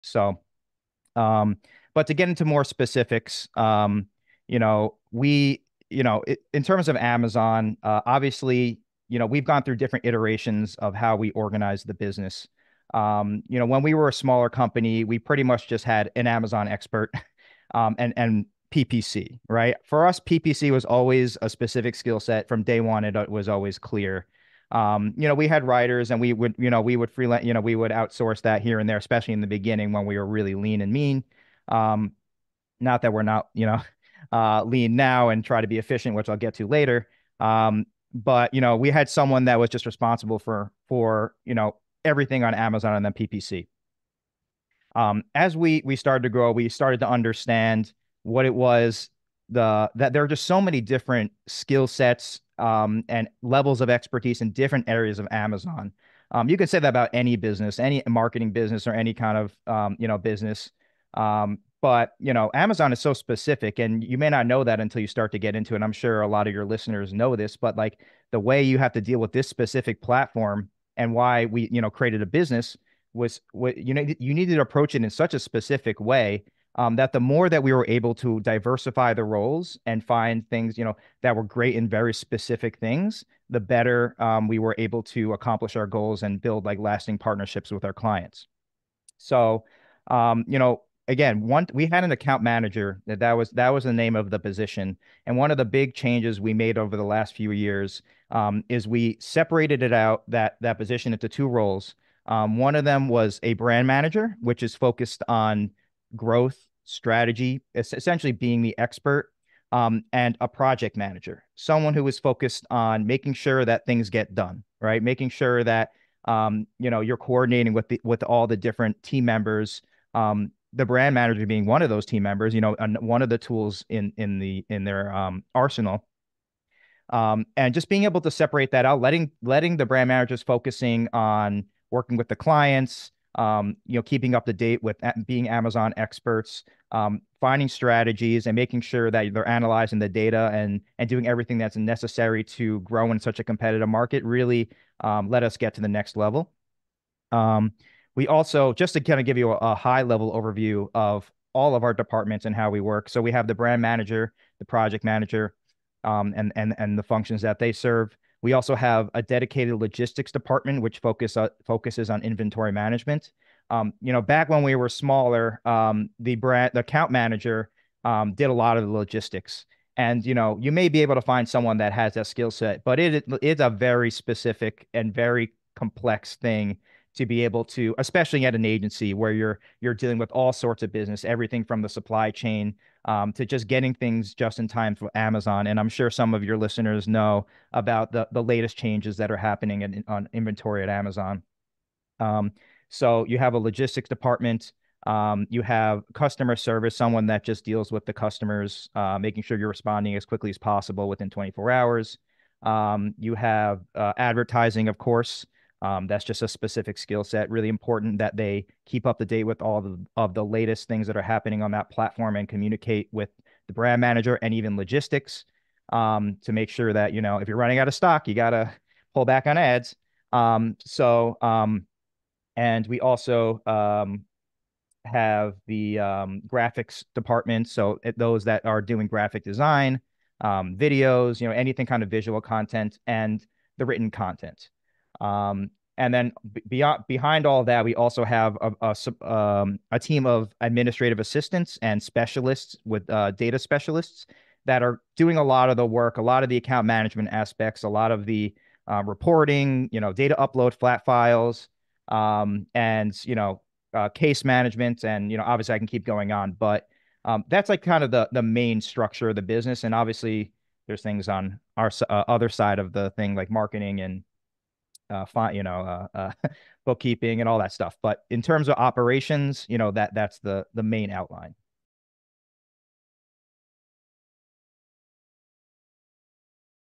Expert Strategies for Efficient Business Operations
Explore the inner workings of our Amazon business in this podcast snippet! 🎙️ Discover how our team structure has evolved to meet the demands of the ever-changing Amazon marketplace. From brand management to logistics, learn about the key roles driving our success. Want to dive deeper? Listen to the full episode on your favorite podcast platform!